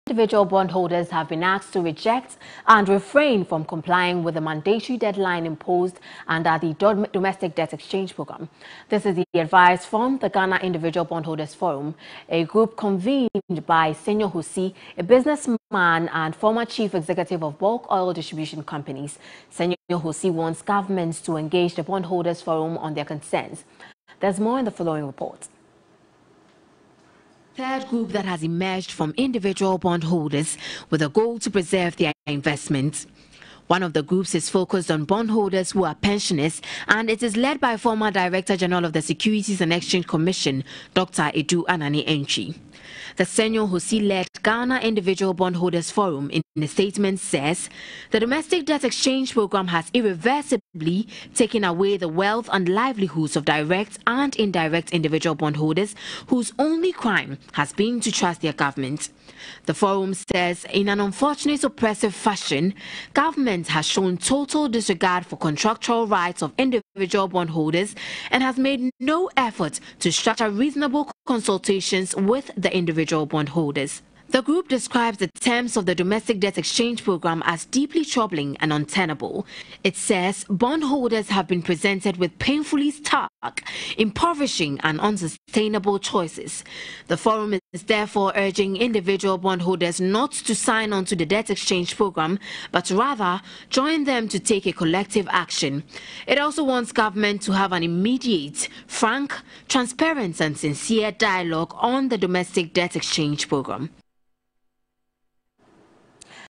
Ghana individual bondholders forum: Groups emerge to preserve bondholders investments.
Groups emerge to preserve bondholders investments.